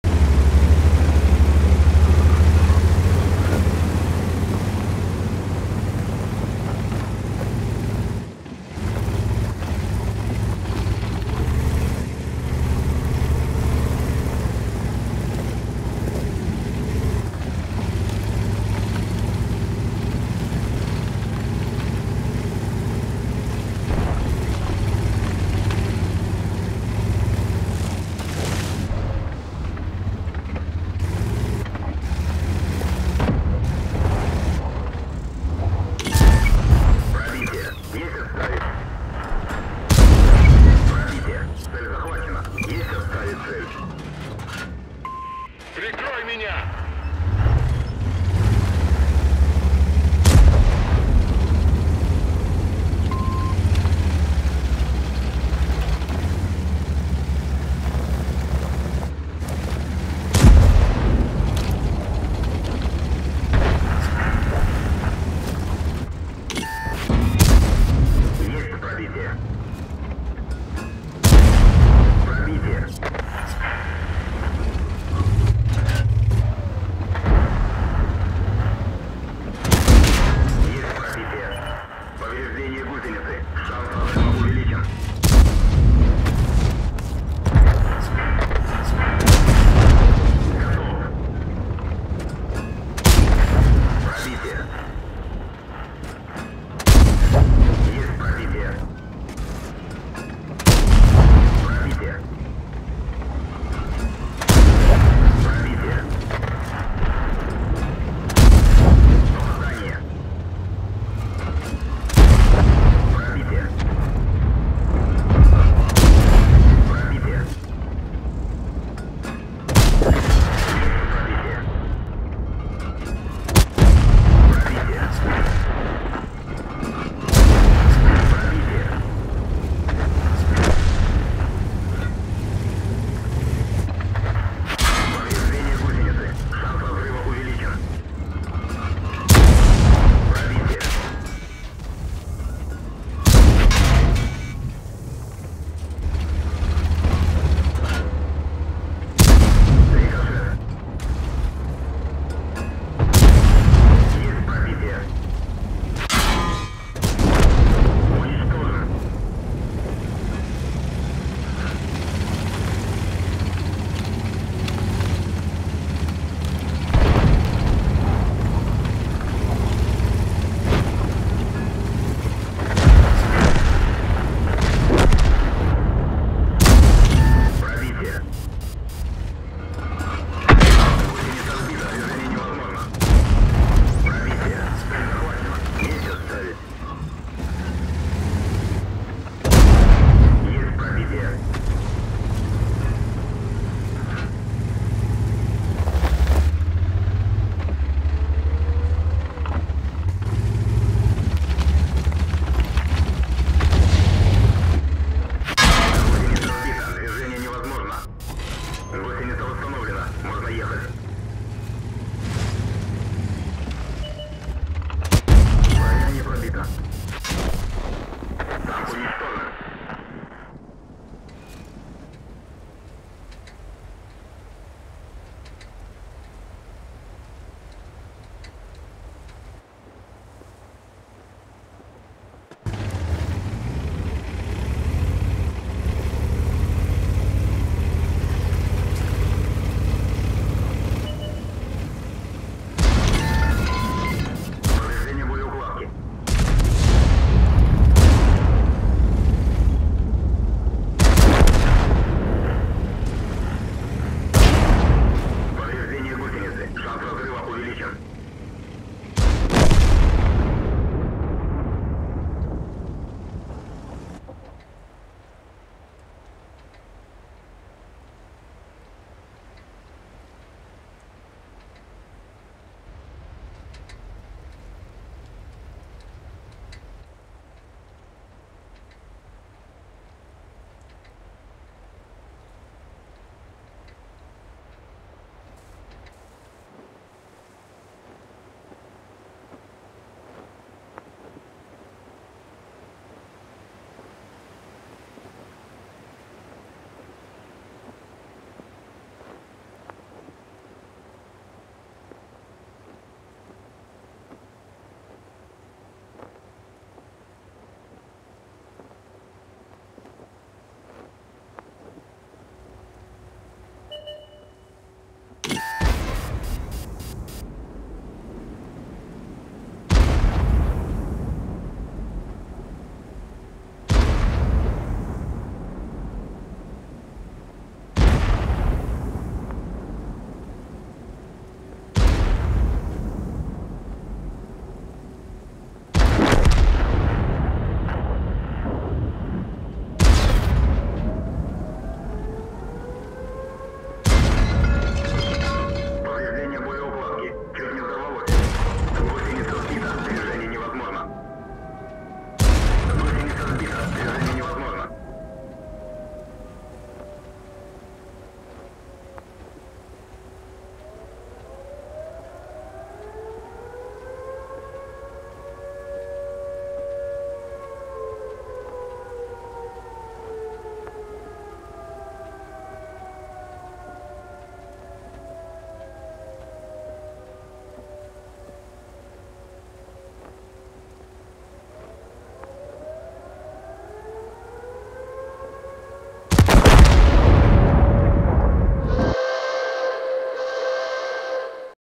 НЕРЕАЛЬНЫЙ скиловик на ЧИФИ!
Реплей игрока - (Funny_Babble)
Танк: T95/FV4201 Chieftain Карта: Перевал.
На новую видеокарту для большего fps и HD для ютуба.